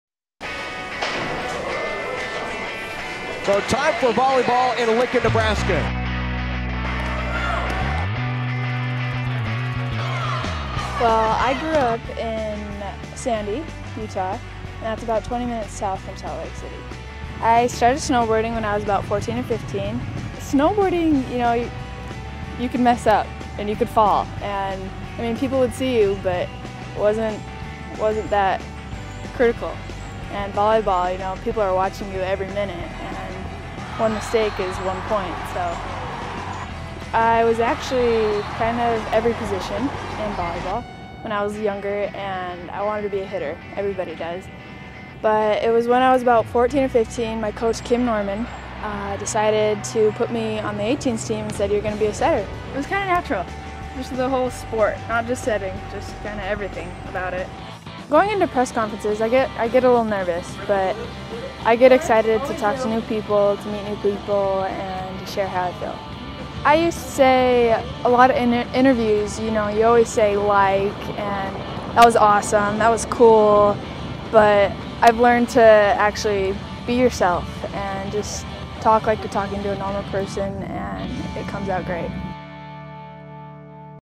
UNL Volleyball - Sydney Anderson
Utah native Sydney Anderson has a found a home in the University of Nebraska Volleyball program. Anderson transfered to the Cornhuskers after starting her college career at Utah. The 2005 Utah Gatorade Player of the Year is now UNL's starting setter and running the offense for one of the most successful teams in college volleyball. In this profile, Anderson talks about adjusting to life in Lincoln, her hobby of snowboarding, and working with the local media.  and click on Sports.